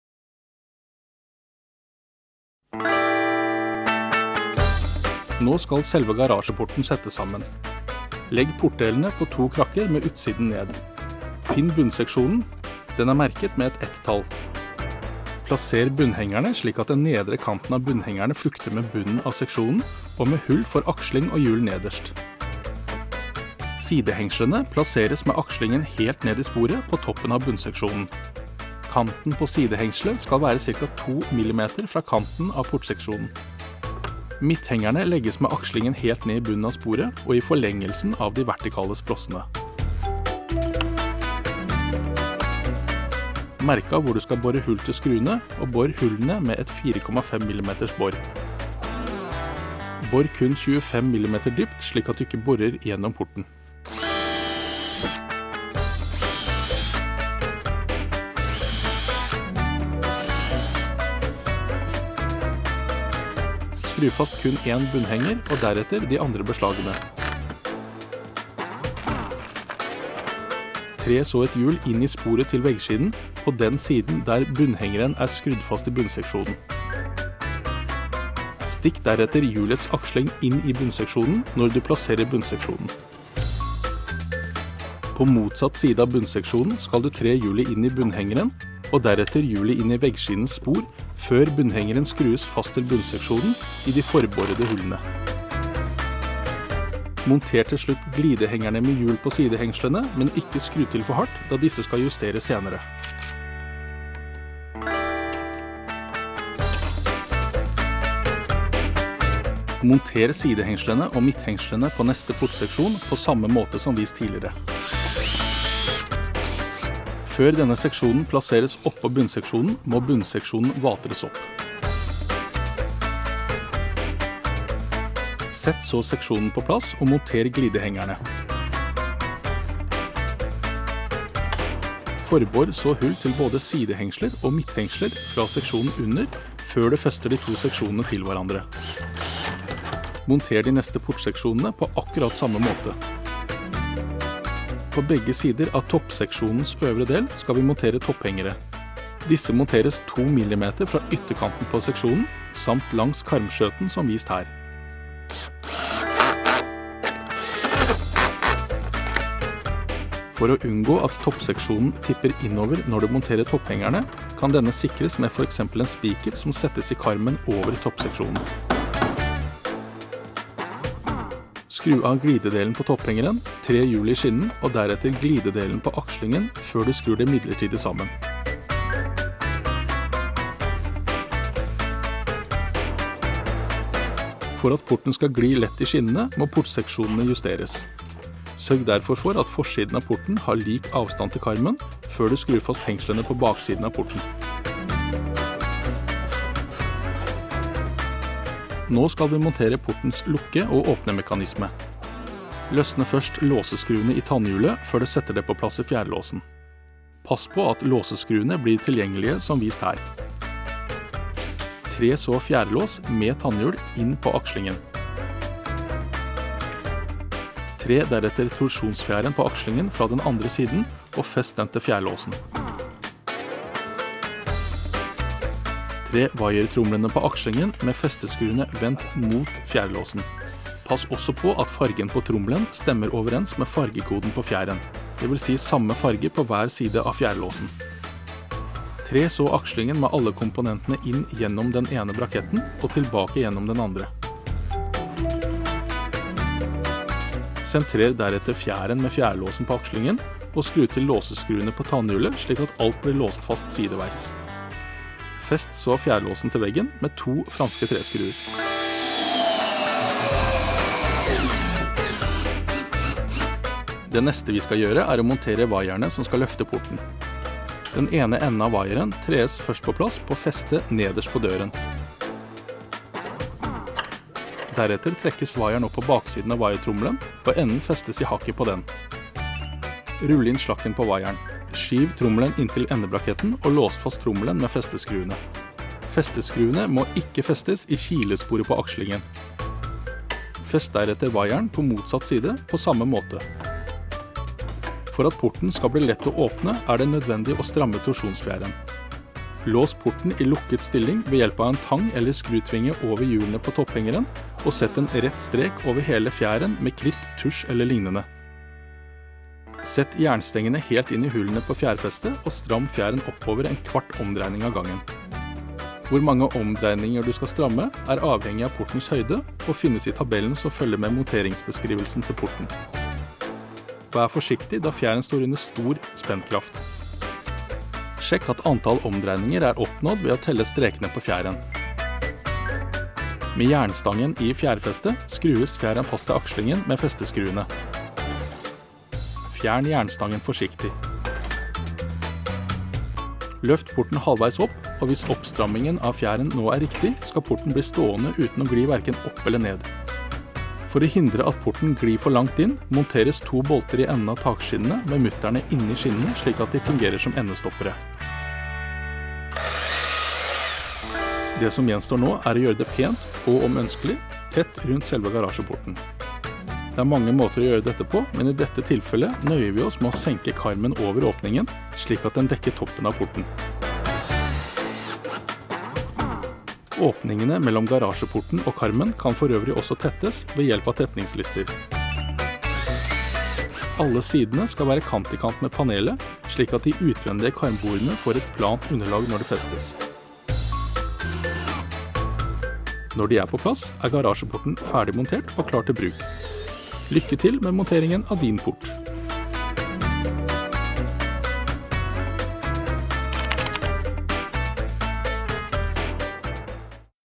Montering av garasjeport 2:2
Del 2 av film om montering av garasjeport. Her monteres selve garasjeporten. Denne filmen er en del av serien om hvordan du bygger en garasje. Besøk våre nettsider